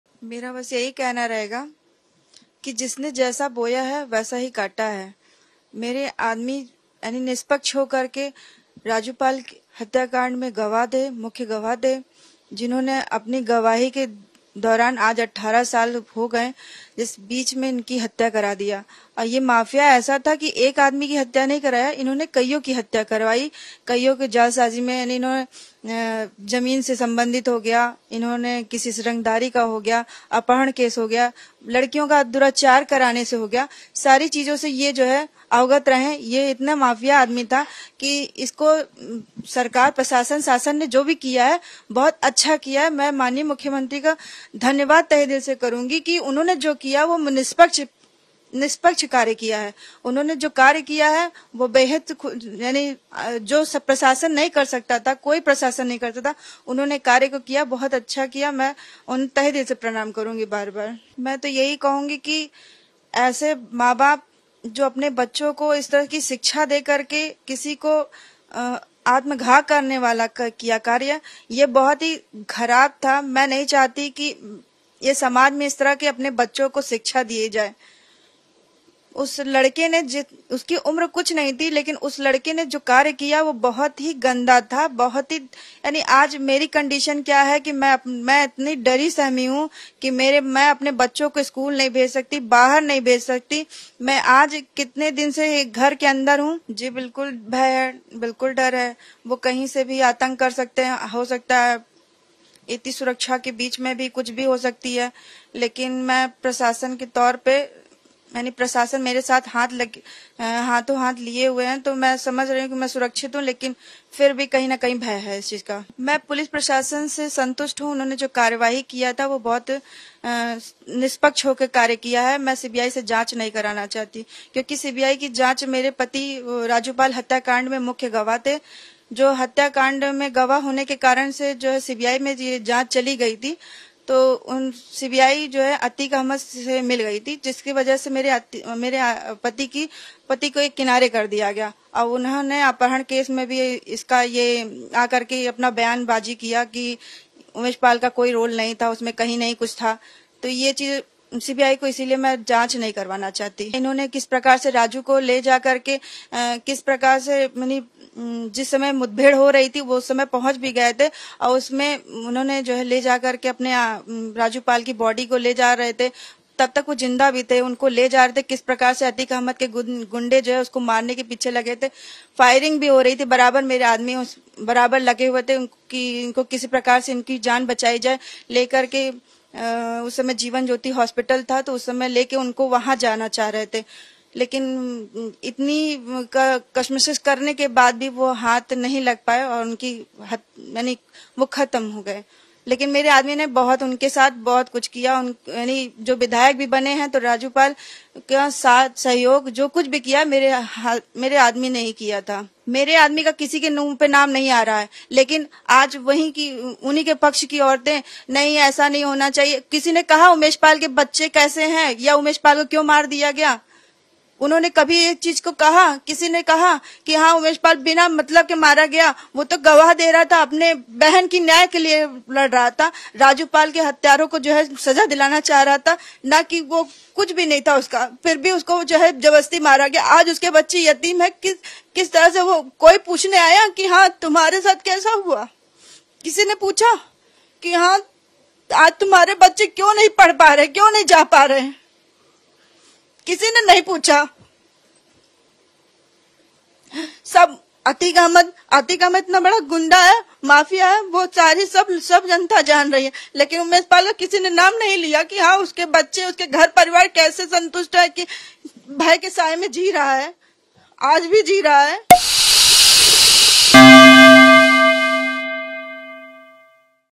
Atique Ahmed की हत्या के बाद पहली बार कैमरे पर Umesh Pal की पत्नी क्यों रो पड़ीं?
First Interview of Umesh Pal's Wife Jaya Pal after Atique Ahmed murder. Jaya told that she can't send her children to school. She also told that she is satisfied with UP police  action over Umesh Pal Murder case and she thanked Uttar Pradesh Cm Yogi Adityanath.